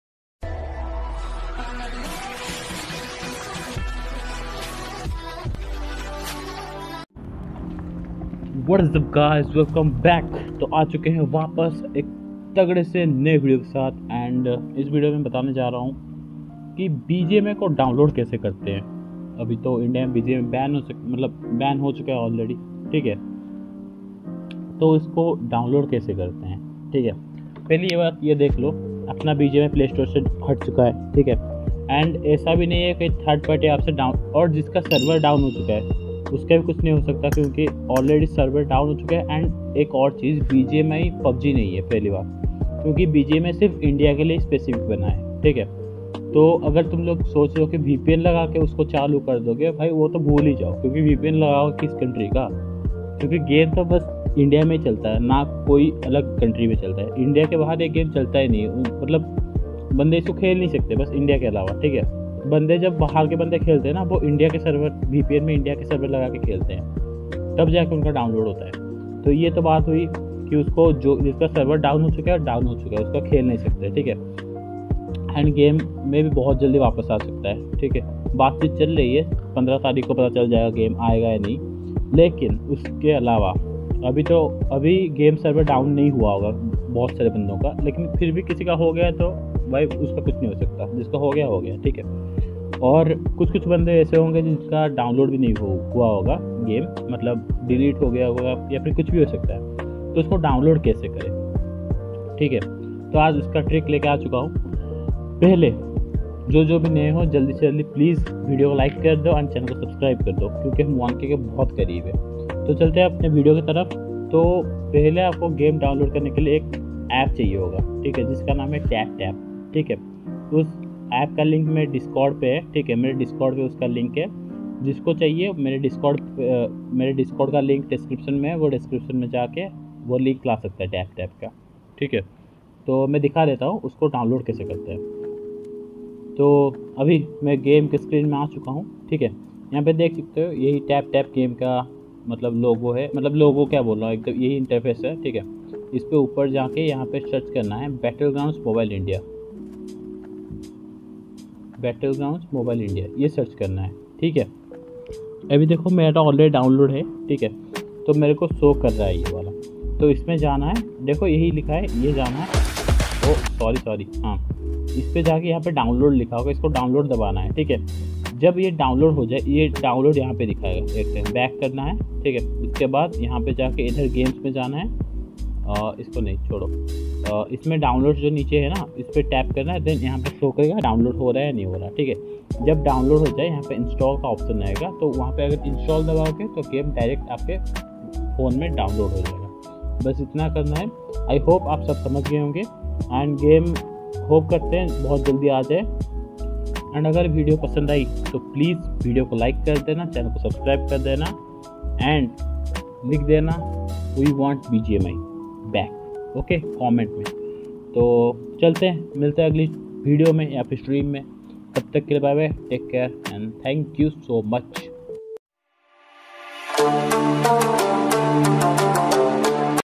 HOW TO DOWNLOAD BGMI AFTER BAN IN INDIA || Captn Dash Yt
Hi Guys in this video i show you how to download bgmi after ban in india